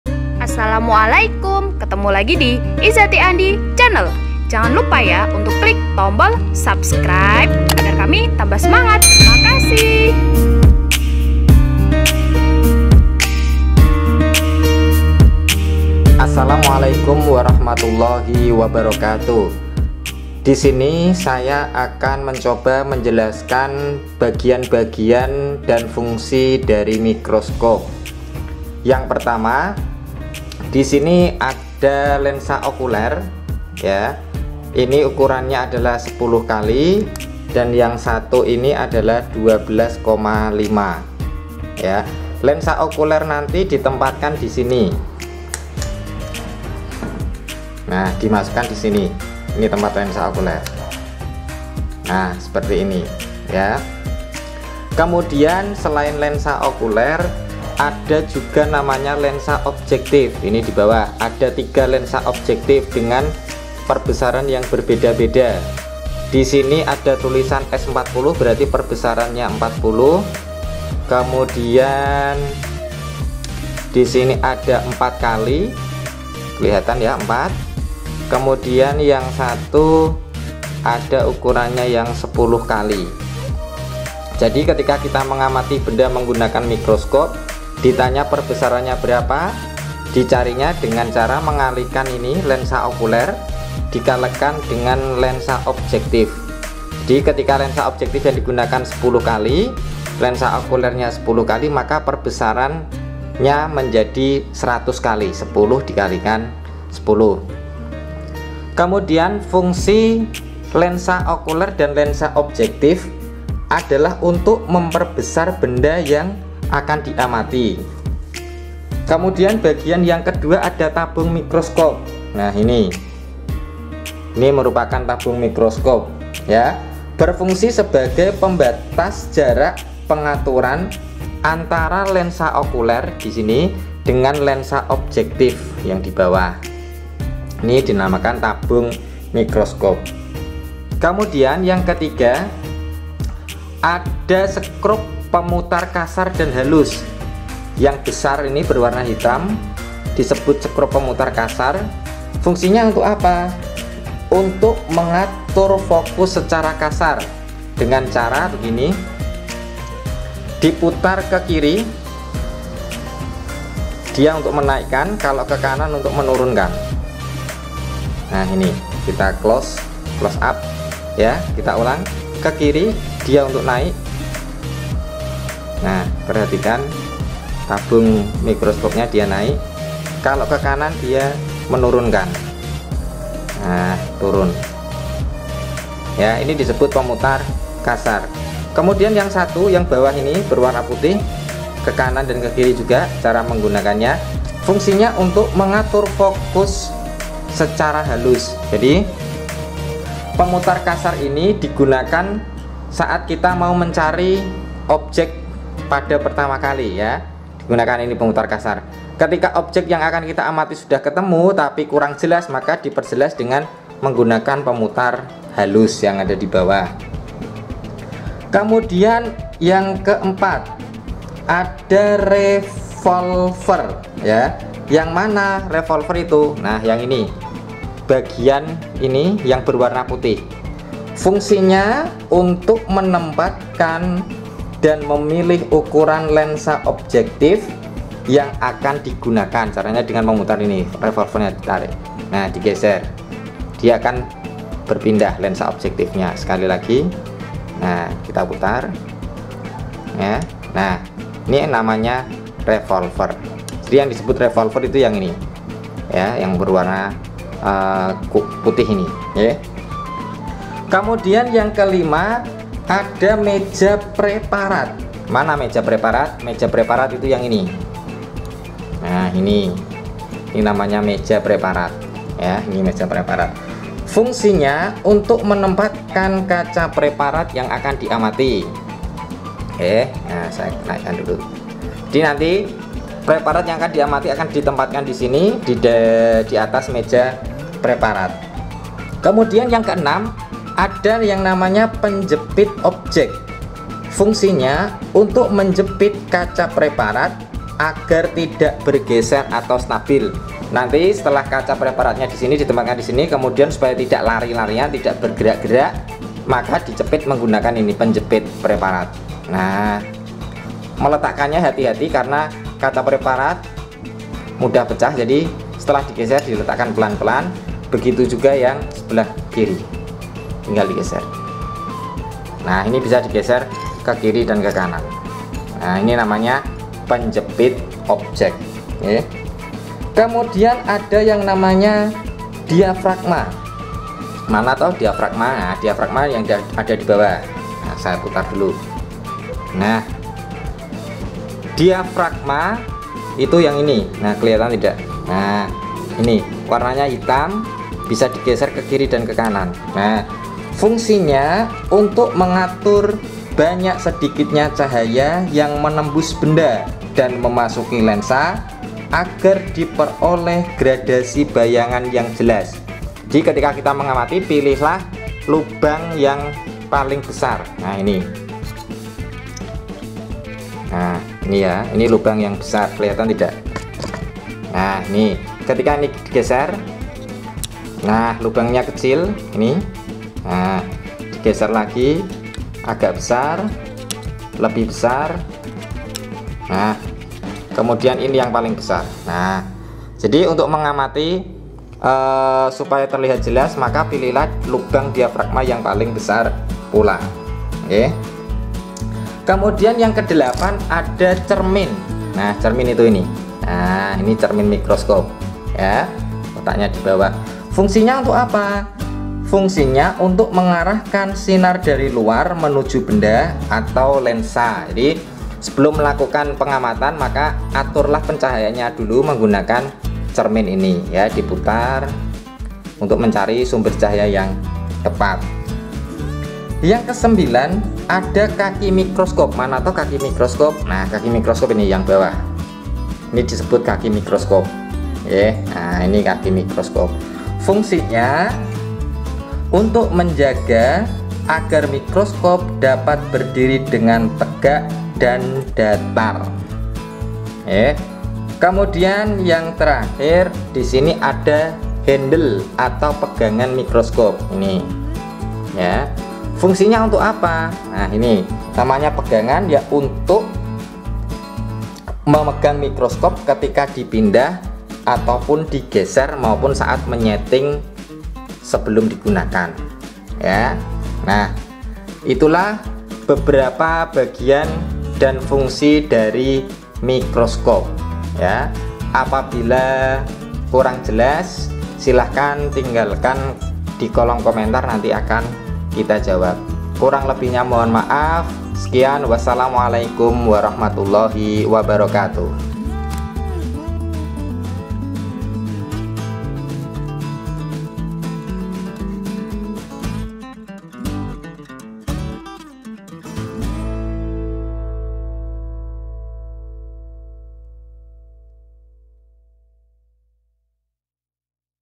BAGIAN-BAGIAN DAN FUNGSI MIKROSKOP
Mikroskop merupakan alat bantu yang memungkinkan kita dapat mengamati obyek yang berukuran sangat kecil. Hal ini membantu memecahkan persoalan manusia tentang organisme yang berukuran kecil.